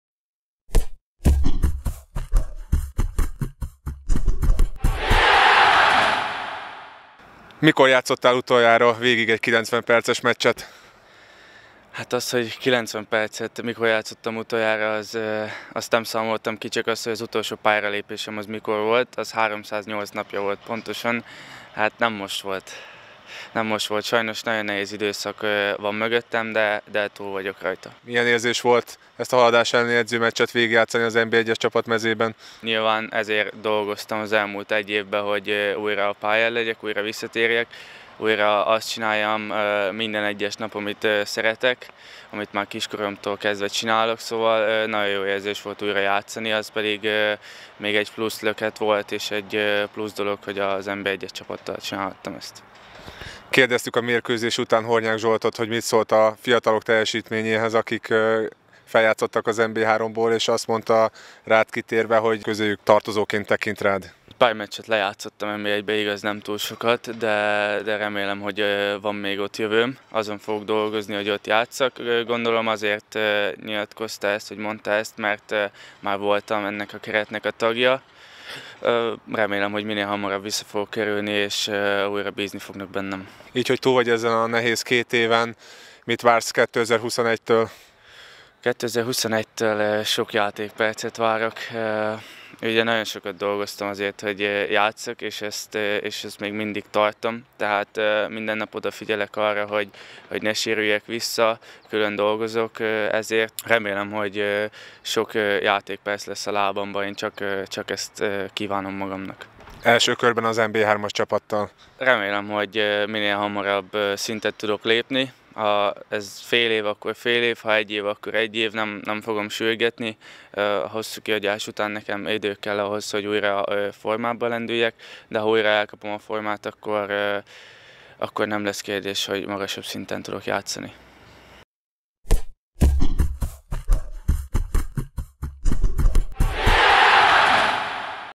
Újra a pályán
Bokros Szilárd 308 nap után lépett pályára újra a sok sérülés után. Külön öröm számára, hogy ezt az első osztályú csapat mezében tehette meg a Haladás elleni (3-0) edzőmeccsen.